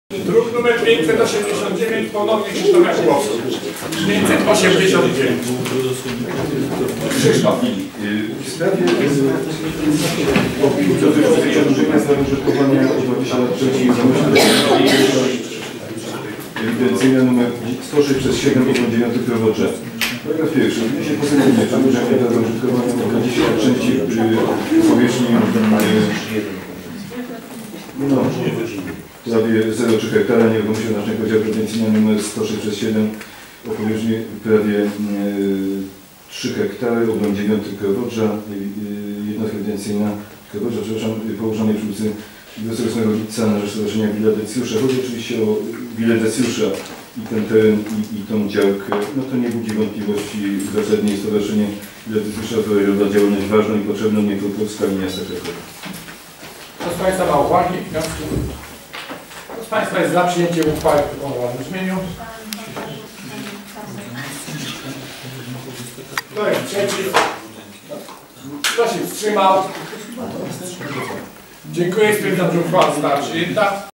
11 opinia dot. przedłużenia prawa użytkowania o 20 lat części nieruchomości
Obrady LI sesji Rady Dzielnicy VII Zwierzyniec z dn. 25 lutego 2014 r.
Uchwała w sprawie opinii dotyczącej przedłużenia prawa użytkowania o 20 lat części nieruchomości oznaczonej jako działka ewidencyjna nr 106/7, obręb 9 Krowodrza.